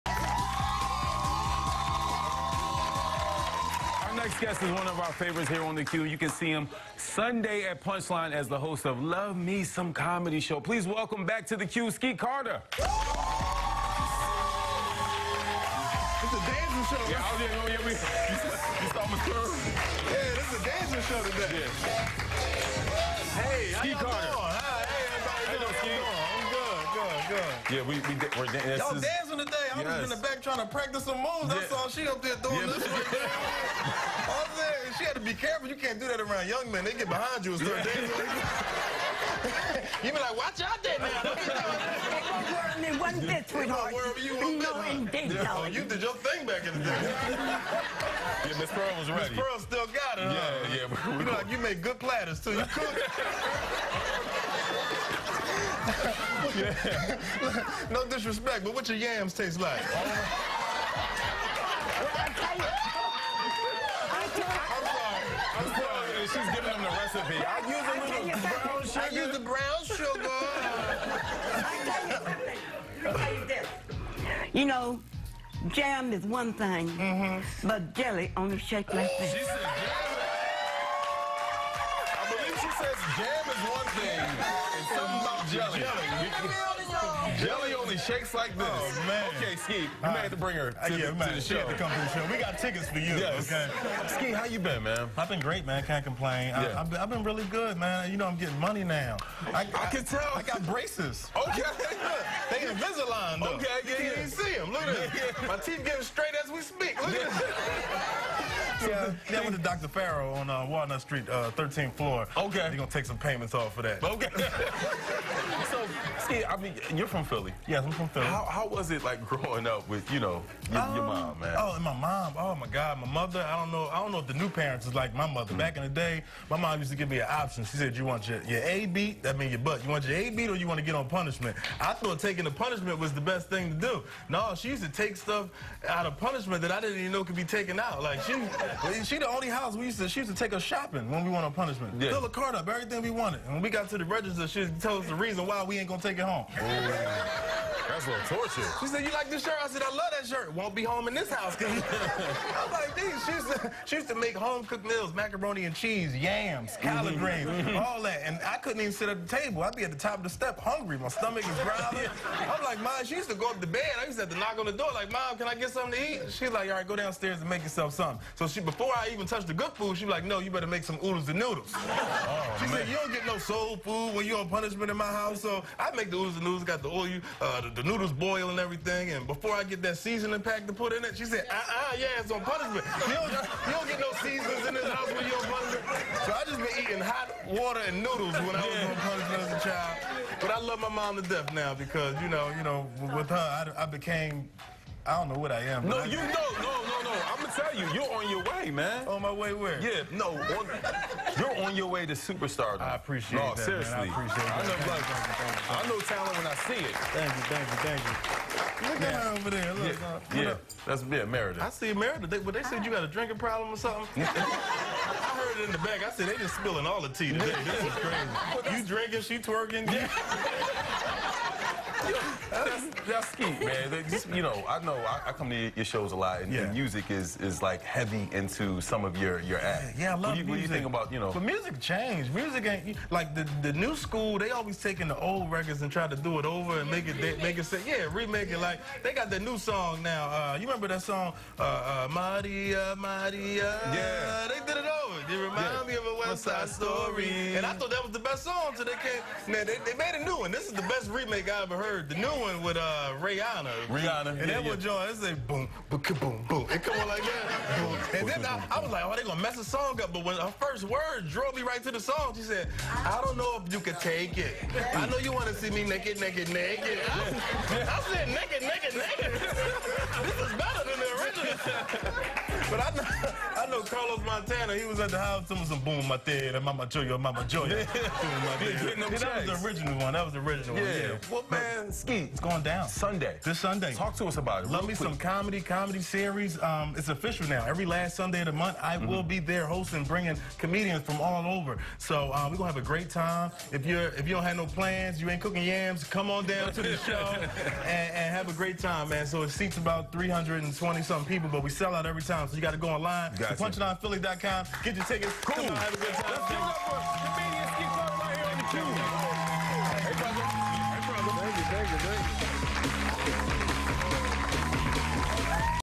Comedian Skeet Carter
Comedian Skeet Carter on The Q.  Skeet Carter talking Parenting and more.